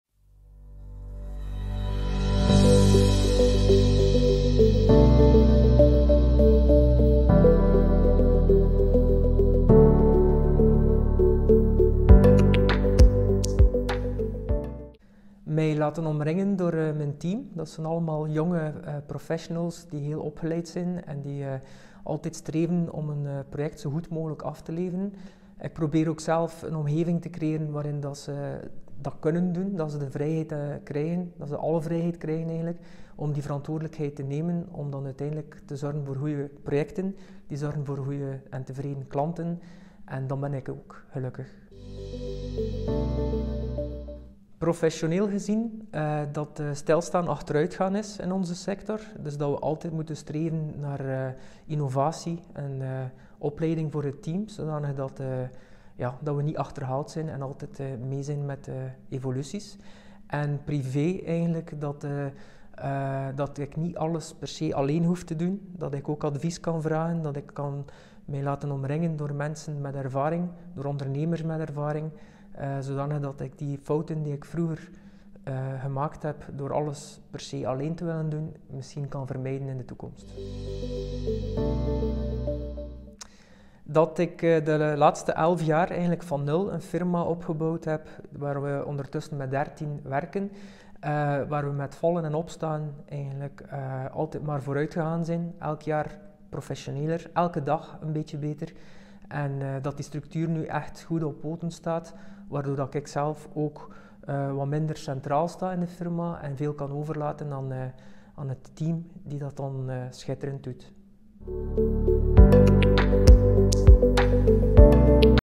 "Stilstaan is achteruitgaan in onze sector" - Gregory Vanhoucke (Exsited)
“Een webproject is geen doel op zich, maar een middel om op verder te bouwen”, dat is de visie van Gregory op de meerwaarde die hij met Exsited biedt. Naast zijn passie voor websites, webshops en webapplicaties is hij een fervente sporter en houdt hij ervan leuke dingen te doen met familie en vrienden.

In onze ledenvideo antwoordt hij op enkele belangrijke vragen …